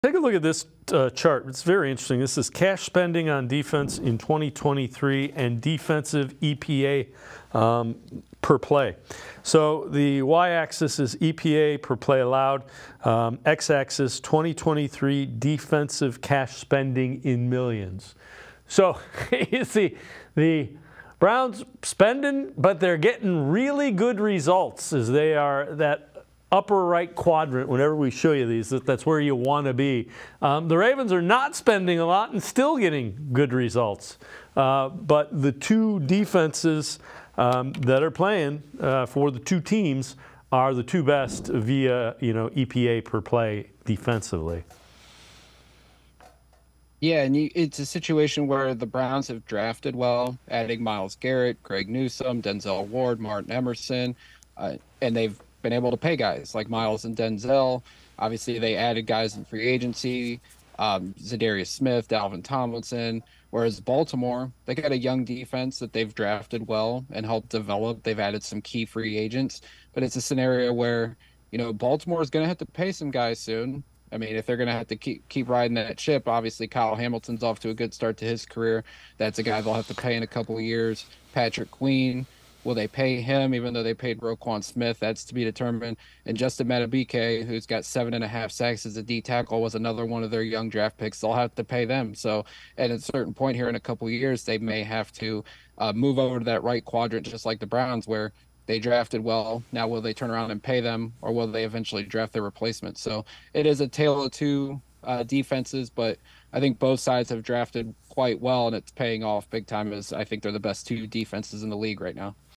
A Key Example of How Andrew Berry Has Spent Wisely for the Browns - Sports4CLE, 11/9/23
Dave Bacon and Jackson McCurry discuss the Browns' spending on defense and the production they have gotten this season.